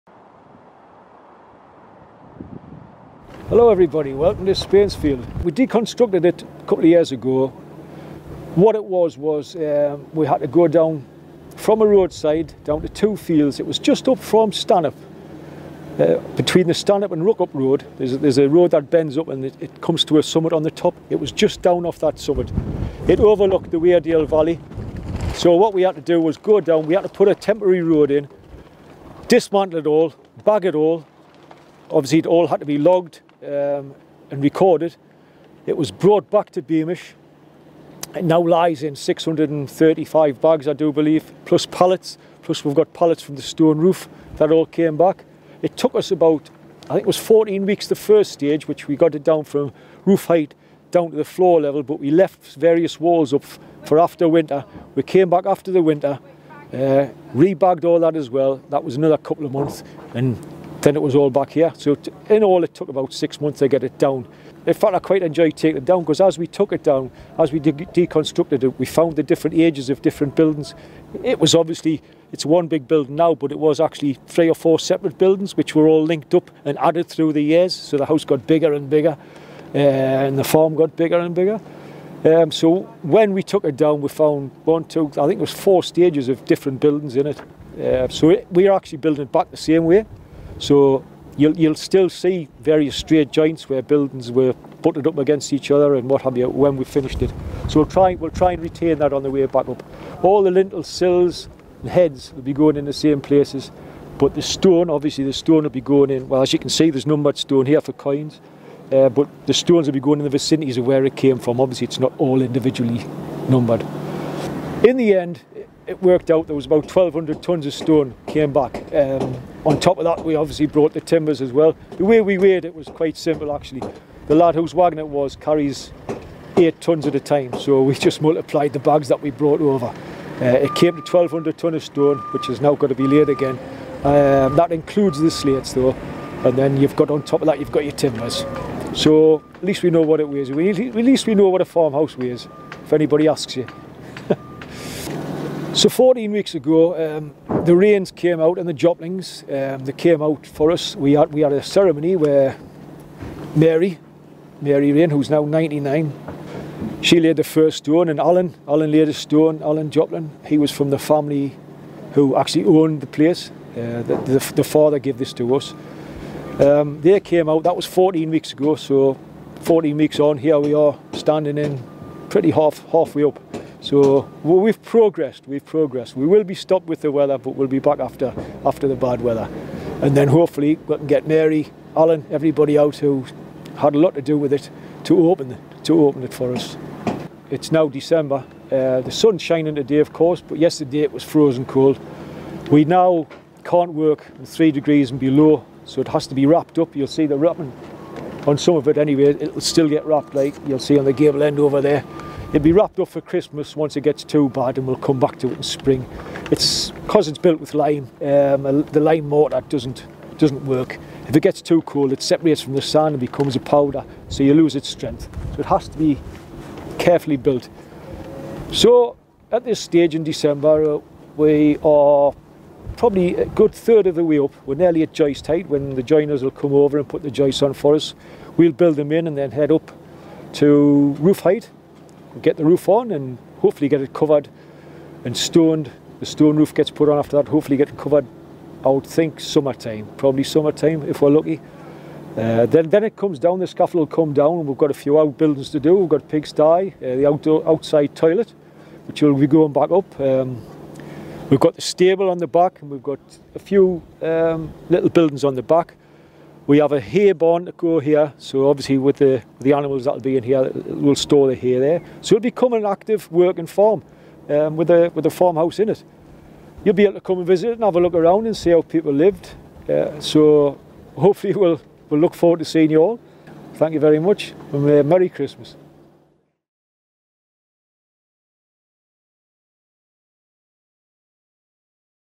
Spain's Field Farm December 2018 update
Spain's Field Farm is being rebuilt stone by stone as part of our Remaking Beamish project.

The Farm, originally located near Stanhope in Weardale was deconstructed in 2016. Following three years of careful recording work, the Team moved 900 tonnes of stone, 50 tonnes of stone roof slates, 60 tonnes of rubble infill and 10 tonnes of timber over a 17 week period!

Paul Marron, Buildings Team Leader (Masonry) provides us with an update on how the reconstruction work is going.

Thanks to the money raised by National Lottery players, the Remaking Beamish project has been awarded £10.9 million by the Heritage Lottery Fund (HLF).